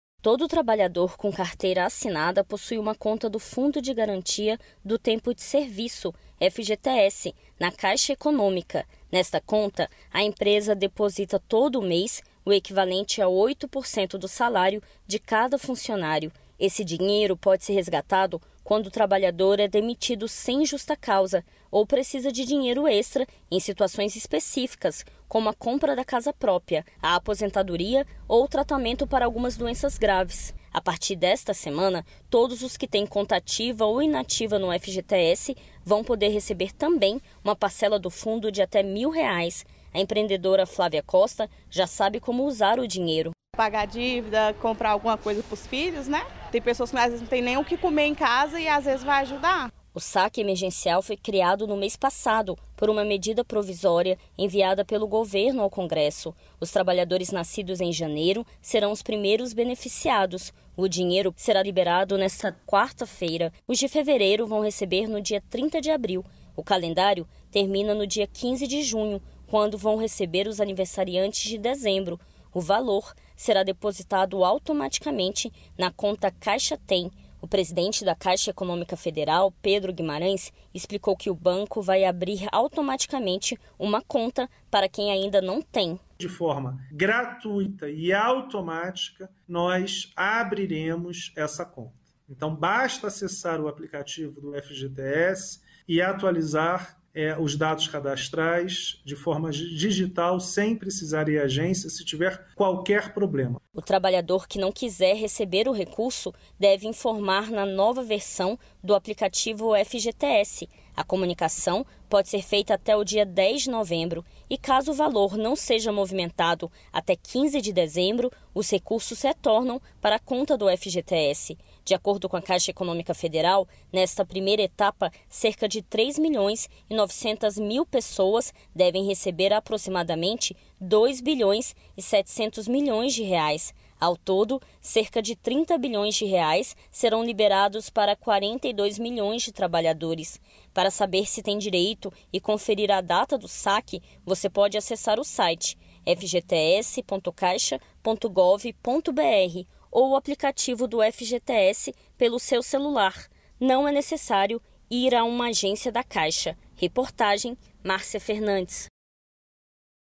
19/04/2022 - COMEÇA NESTA QUARTA-FEIRA PAGAMENTO DO SAQUE EMERGENCIAL DO FGTS | Matérias da Voz
O crédito de até R$ 1.000,00 por trabalhador vai ser feito automaticamente na conta Caixa Tem. Quase 4 milhões de correntistas do FGTS recebem nesta primeira etapa.

Reportagem: Márcia Fernandes.

registrado em